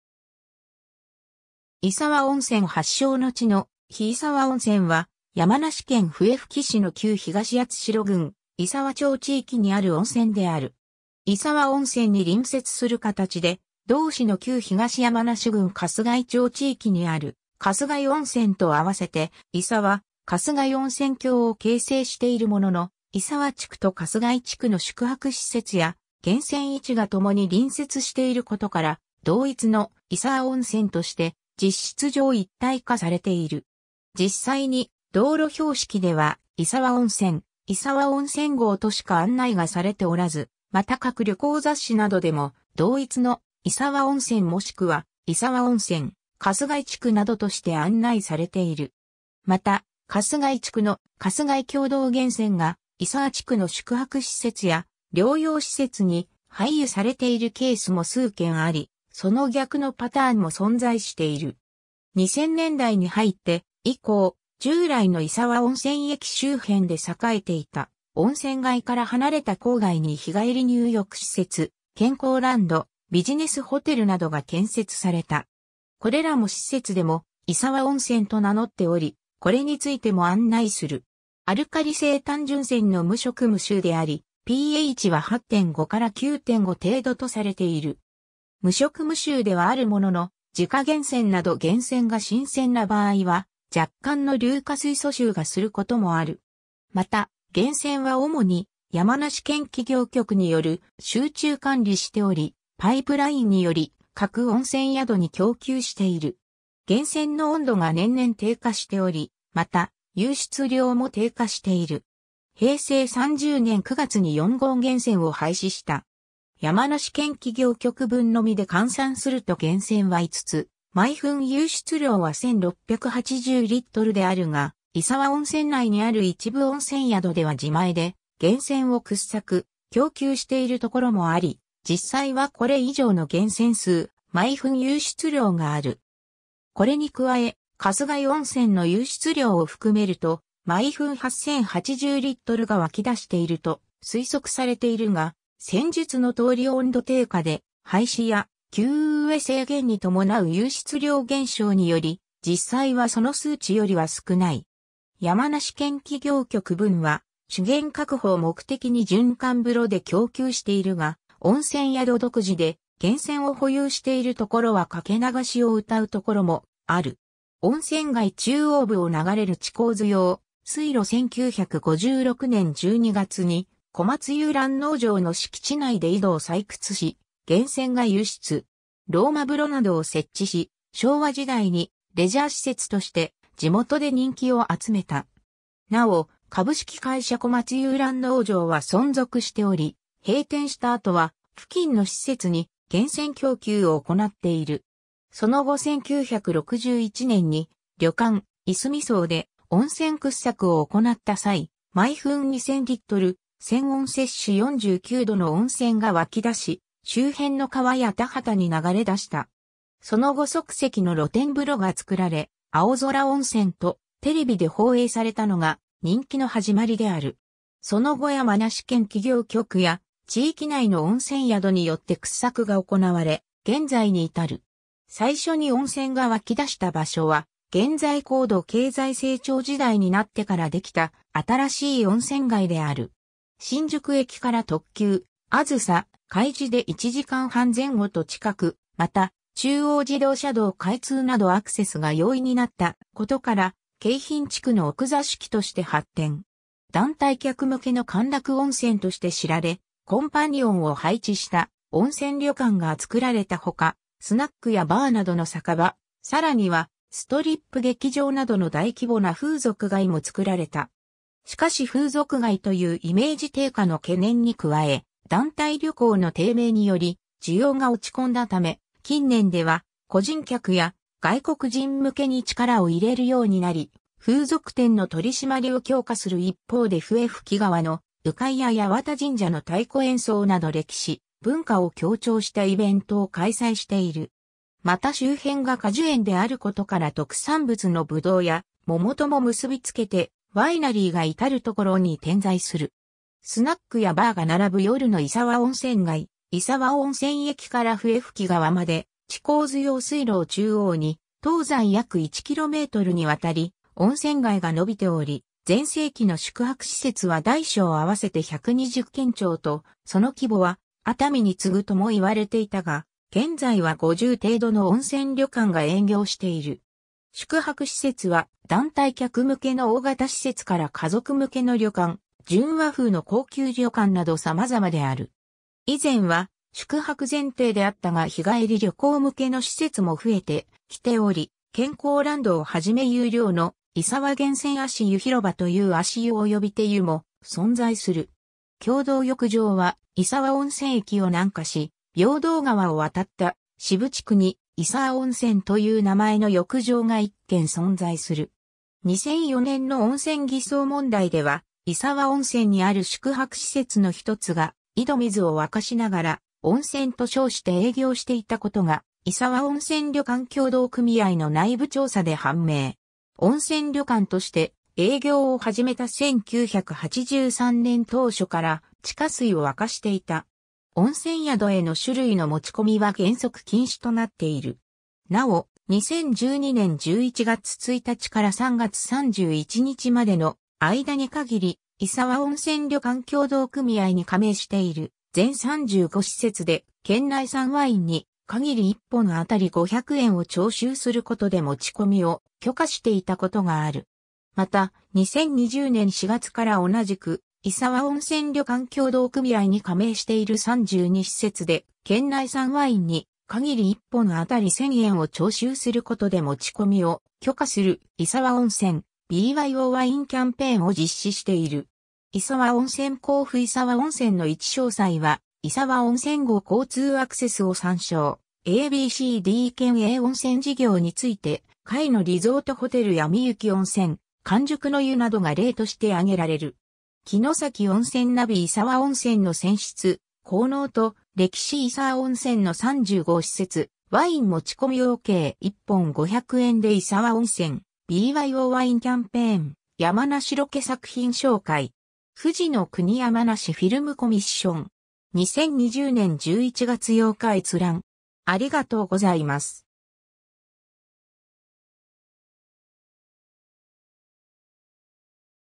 石和温泉
石和温泉（いさわおんせん）は、山梨県笛吹市の旧東八代郡石和町地域にある温泉。
石和温泉に隣接する形で、同市の旧東山梨郡春日居町地域にある春日居温泉と合わせて、石和・春日居温泉郷を形成する。
 石和温泉 甲府 石和温泉の位置 石和地区と春日居地区の宿泊施設や源泉位置が共に隣接していることから、同一の温泉街「石和温泉郷」として実質上一体化している。
実際に道路標識では「石和温泉」「石和温泉郷」としか案内がされておらず、旅行雑誌などでも同一の「石和温泉」もしくは「石和温泉・春日居地区」などとして案内されている。
春日居地区の「春日居共同源泉」が、石和地区の宿泊施設や療養施設に配湯されているケースも数軒あり、その逆のパターンも存在する。
 2000年代に入って以降、石和温泉駅周辺で栄えていた従来の温泉街から離れた郊外に、日帰り入浴施設や健康ランド、ビジネスホテルなどが建設された。
これらの施設でも「石和温泉」と名乗っており、これについても記述する。
 「石和温泉郷」も参照 「石和温泉発祥之地」の碑（ウインズ石和） スナックやバーが並ぶ夜の石和温泉街（2013年8月2日撮影） 1956年（昭和31年）12月、小松遊覧農場（2020年現在は日本中央競馬会の場外勝馬投票券発売所「ウインズ石和」）の敷地内で井戸を採掘したところ源泉が湧出。
ローマ風呂などを設置し、昭和時代にレジャー施設として地元で人気を集めた。
なお、株式会社小松遊覧農場は存続しており、閉店した後は付近の施設に源泉供給を行っている。
「ウインズ石和」の入口付近には「石和温泉発祥之地」の碑が建っている。
 1961年（昭和36年）に旅館「いすみ荘」で温泉掘削を行った際、毎分2,000L、泉温49℃の温泉が湧出し、周辺の川や田畑に流れ出した。
即席の露天風呂が作られ「青空温泉」とテレビで放映されたのが人気のはじまりである。
その後、山梨県企業局や地域内の温泉宿によって掘削が行われ現在に至る。
 戦後の高度成長期に成立した新しい温泉街で、歴史が浅く街並みなどの情緒に乏しい反面、京浜地区から近く交通アクセスが良好で、新宿駅から特急「あずさ・かいじ」で約1時間半、さらに中央自動車道が開通したことから、歓楽温泉街として発展した。
 団体旅行客向けの歓楽温泉として知られ、コンパニオンを配置し「ピンクコンパニオンプラン」を売りにした温泉旅館が作られたほか、スナックやバーなどの酒場、さらにはストリップ劇場などの大規模な風俗街も作られた。
しかしバブル崩壊後は団体旅行の低迷により需要が落ち込んだ上、風俗街・歓楽街の印象が強くなり観光地としてのブランドイメージが低下した。
 そのため近年では、個人客や外国人向けに力を入れるようになり、風俗店の取り締まりを強化する一方で、笛吹川の鵜飼いや八幡神社の和太鼓演奏など、歴史・文化を強調したイベントを開催している。
また周辺に果樹園やワイナリーが多いことから特産物のブドウやモモと結びつけ、ワインや観光農園でのフルーツ狩りをPRしている。
 温泉街中央部を流れる近津用水路（2013年8月2日撮影） 石和温泉駅から笛吹川まで近津用水路を中央に、東西約1kmにわたり温泉街が伸びている。
 全盛期の宿泊施設は大小合わせて120軒超と、その規模は熱海に次ぐとも言われていたが、現在は50程度の温泉旅館が営業している。
 宿泊施設は団体客向けの大型施設から家族向けの旅館、純和風の高級旅館など様々である。
以前は宿泊前提であったが、日帰り旅行向けの施設も増えてきており、健康ランドをはじめ有料の「石和源泉足湯ひろば」という足湯及び手湯も存在する。
 共同浴場は石和温泉駅を南下し、平等川を渡った市部地区に「石和温泉」という名前の浴場が1件存在する。
 「石和温泉郷#交通アクセス」を参照 アルカリ性単純泉の無色無臭であり、pHは8.5～9.5程度とされる。
無色無臭ではあるものの、自家源泉など源泉が新鮮な場合は若干の硫化水素臭がすることもある。
 源泉は主に山梨県企業局により集中管理されており、パイプラインにより各温泉宿に供給している。
源泉の温度が年々低下しており、また湧出量も低下している。
平成30年9月に4号源泉を廃止した。
 山梨県企業局分のみで換算すると源泉は5つ、毎分湧出量は1,680Lであるが、石和温泉内にある一部温泉宿では自前で源泉を掘削・供給している所もあり、実際はこれ以上の源泉数・毎分湧出量がある。
これに加えて春日居温泉の湧出量を含めると毎分8,080Lが湧出...